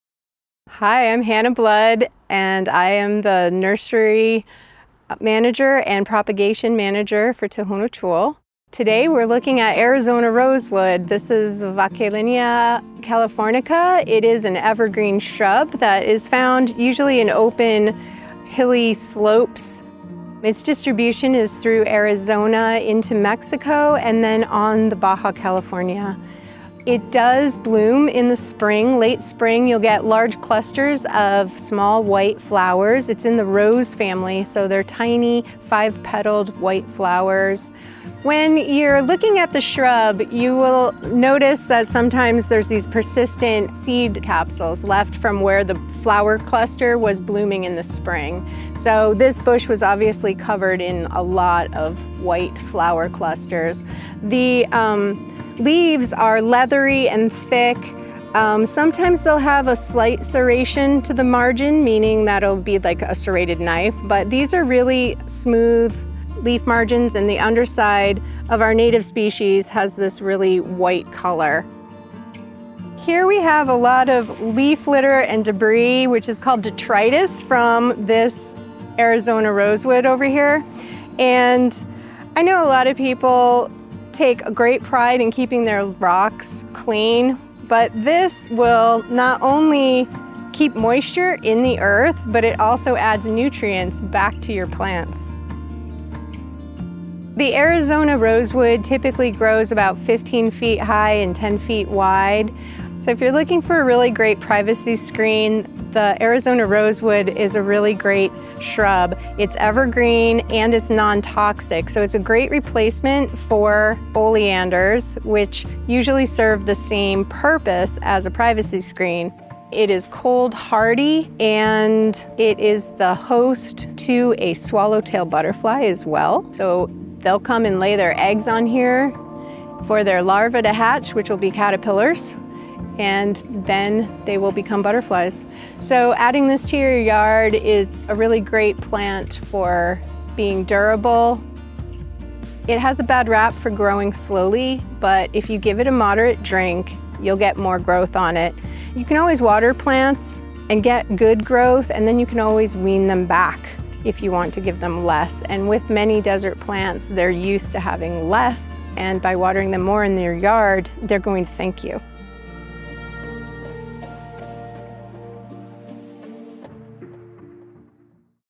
Desert Plants: Arizona Rosewood
The Arizona Rosewood, Vauquelinia californica, is a drought and cold-tolerant native plant of the Sonoran Desert that makes a good choice for a home landscape since it can be grown as a natural hedge to replace oleanders, which are non-native and toxic to people and animals.

Producer: Tony Paniagua
Videographers: Nate Huffman
Editor: Nate Huffman